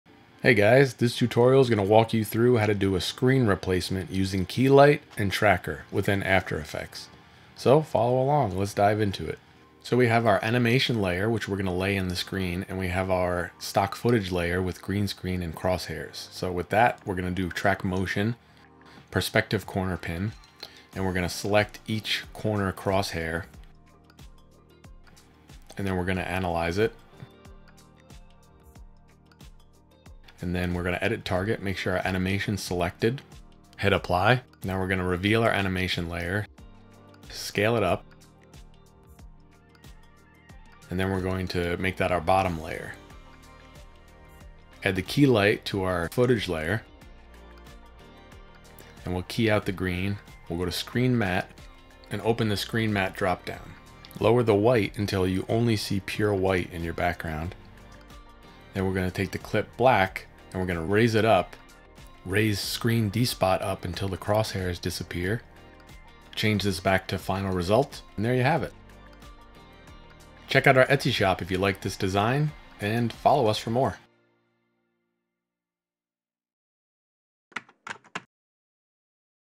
After Effects Tutorial: Keylight Effect Tracker Screen Replacement
Exclusive Courses, Free Downloads & More ➜ LinesToDesigns.com
Learn how to superimpose your image or video onto a device with a green screen using the keylight effect and the built-in tracker in After Effects. This simple Screen Replacement tutorial will give you plenty of flexibility when working with your animations and motion graphic designs within After Effects.